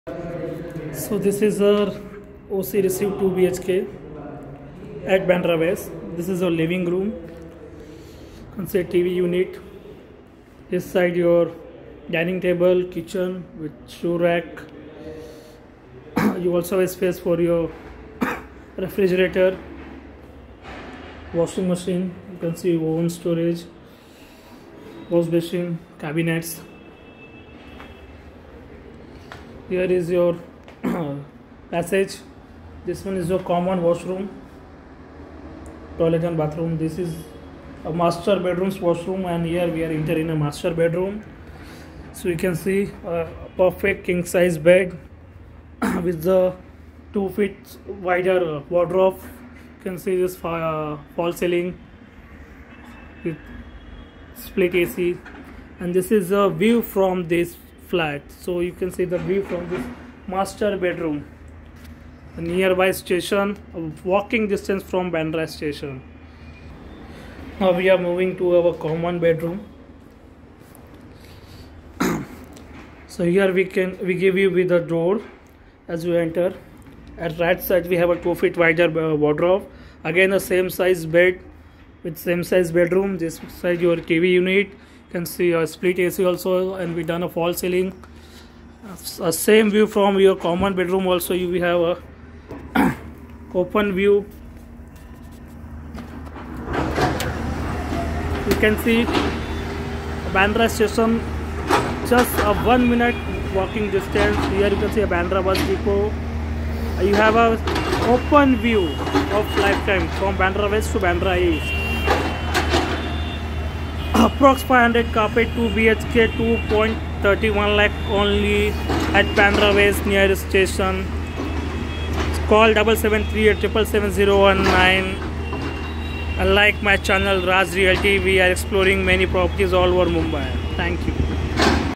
2 BHK OC RECEIVED BANDRA WEST AT 2.31 CR ONLY..
Star Sayba Emerald is one of the residential developments of "Star Regency. It offers spacious and skilfully designed 1BHK, 2BHK apartments. The project is well equipped with all the basic amenities to facilitate the needs of the residents.